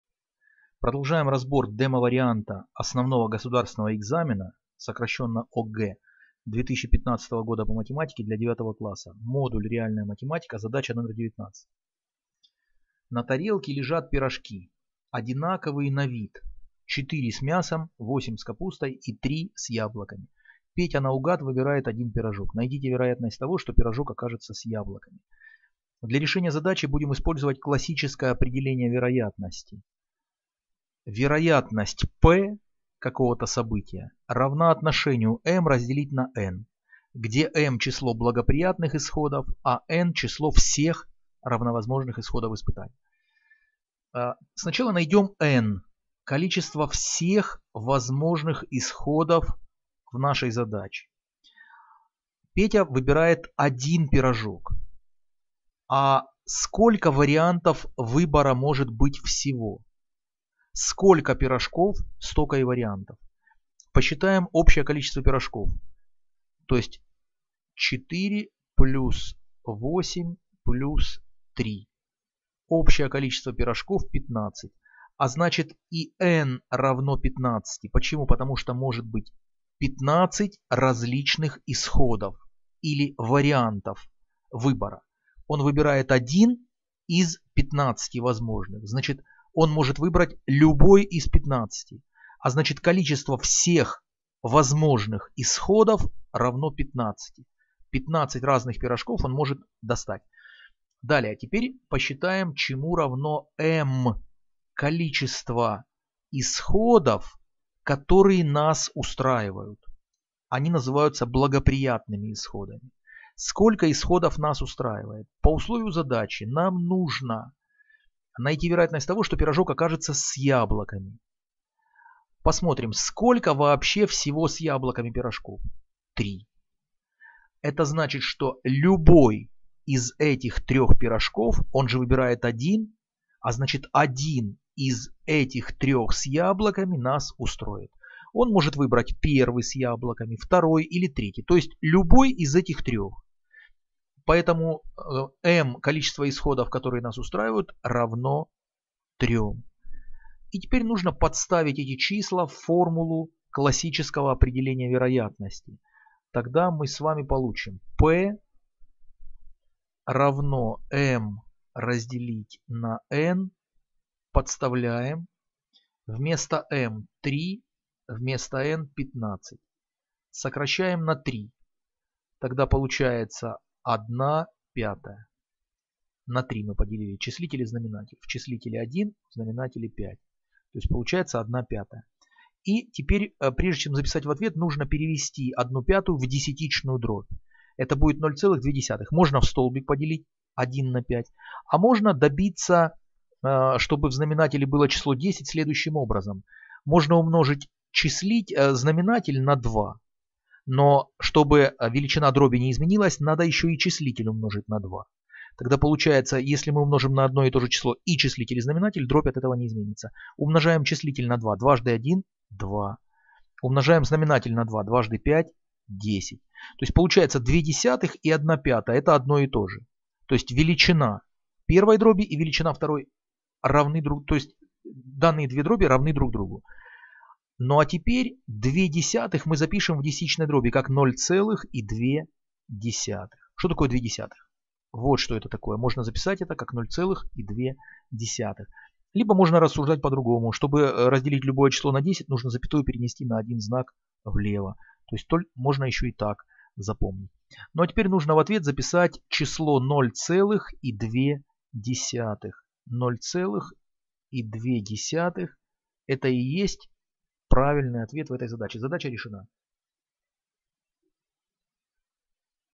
Демо вариант ОГЭ по математике, задача 19
Демо ОГЭ задание 19.
На тарелке лежат пирожки, одинаковые на вид:  4 с мясом, 8 с капустой и 3 с яблоками. Петя наугад выбирает один пирожок. Найдите вероятность того, что пирожок окажется с яблоками.
Webmoney:
WMR-кошелёк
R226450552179
WMZ-кошелёк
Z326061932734
WME-кошелёк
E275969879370